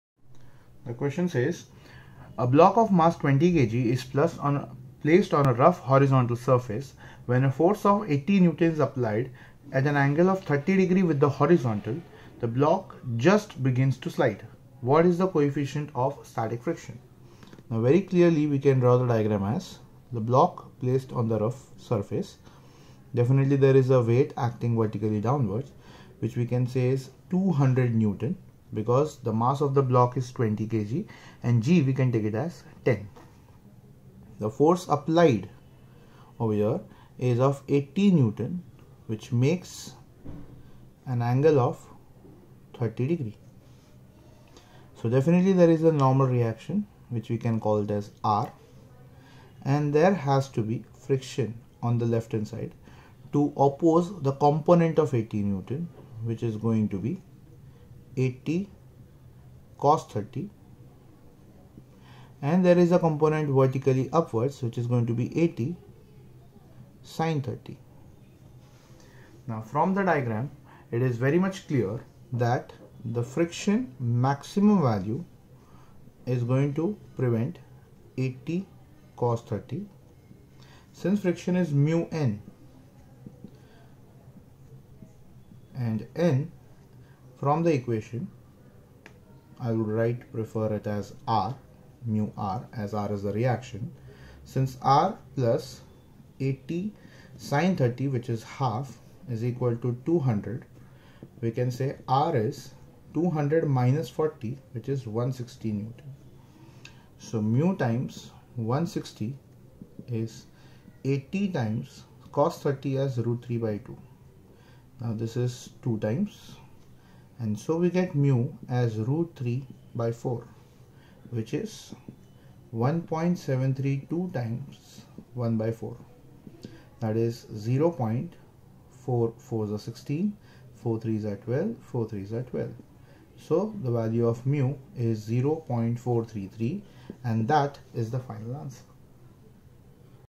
A block of mass 20kg is placed on a rough horizontal surface. When a force of 80N is applied at...
A block of mass 20kg is placed on a rough horizontal surface. When a force of 80N is applied at an angle of 30 with the horizontal, the block just begins to slide. What is the coefficient of static friction?